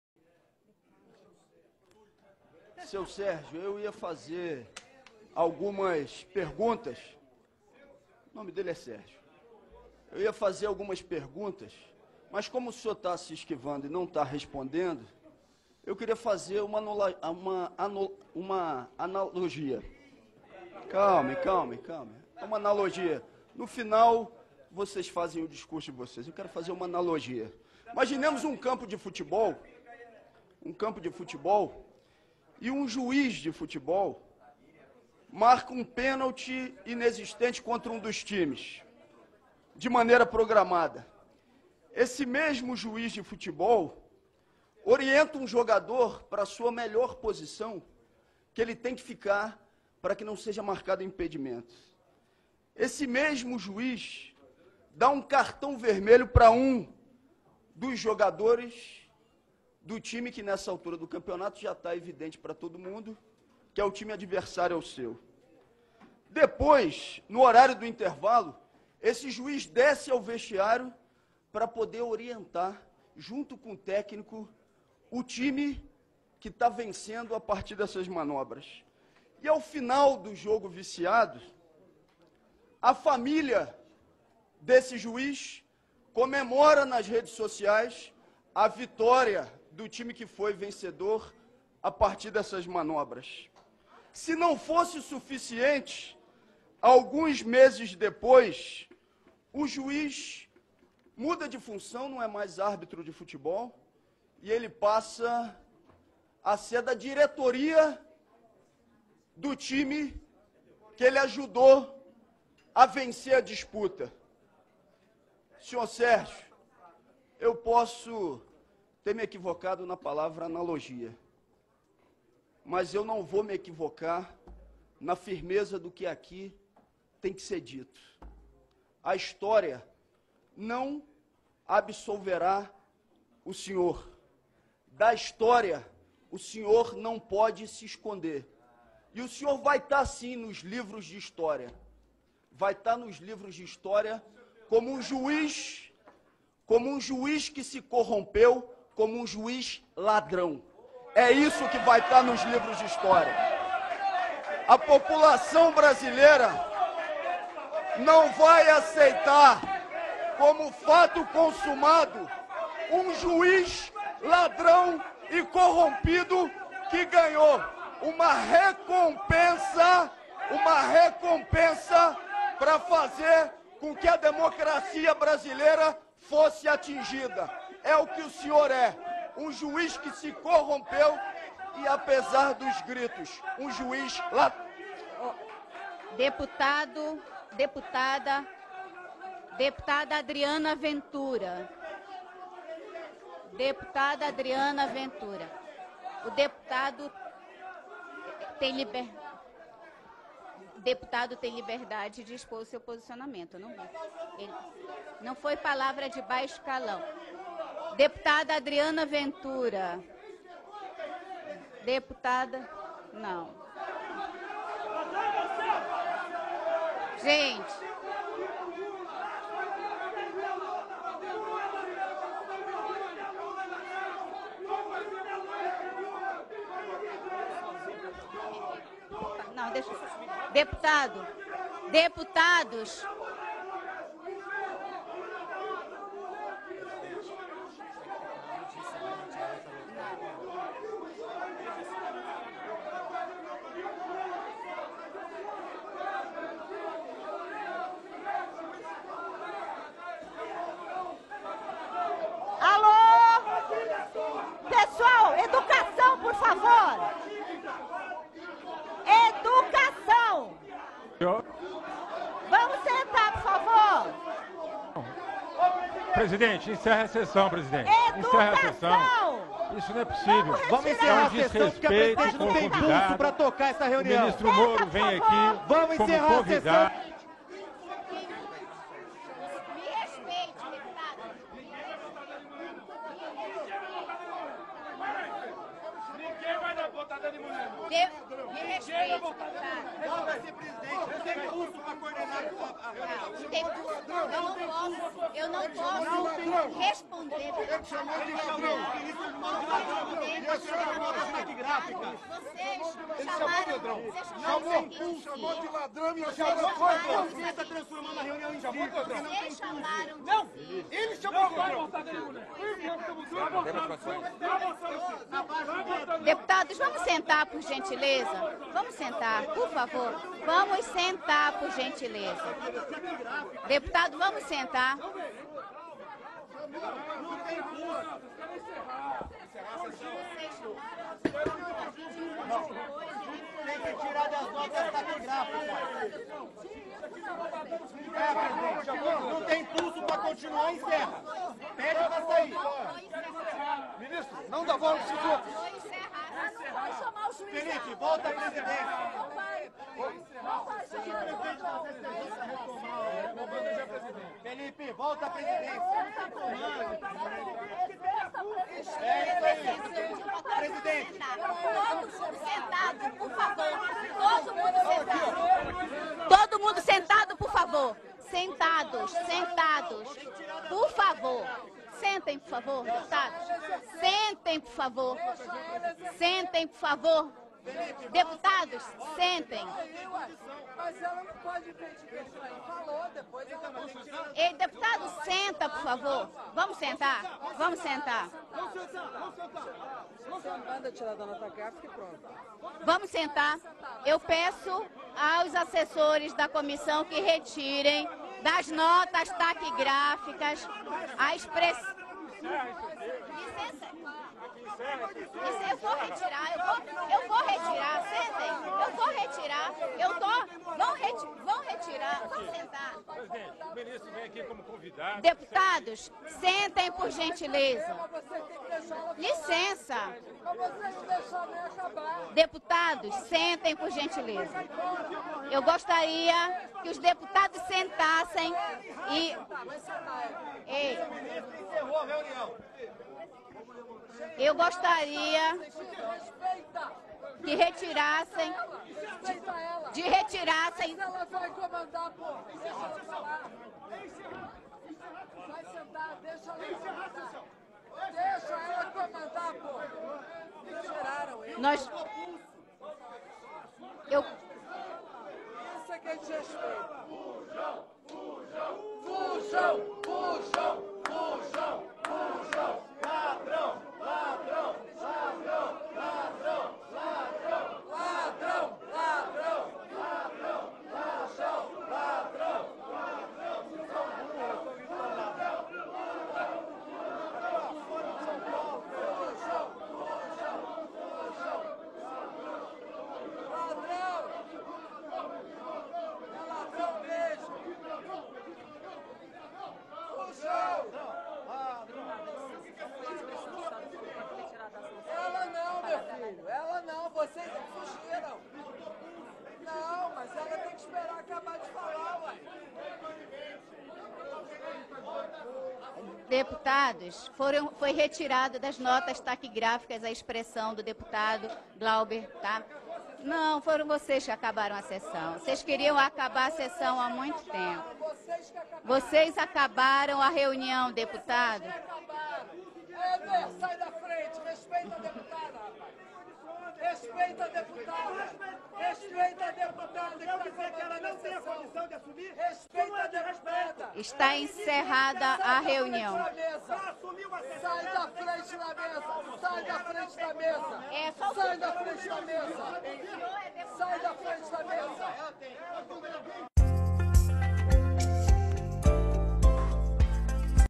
Glauber diz a Sergio Moro que é um juiz ladrão
Glauber diz para Moro o que muita gente queria dizer:
“Juiz ladrão e corrompido que ganhou uma recompensa para fazer com que a democracia brasileira fosse atingida”.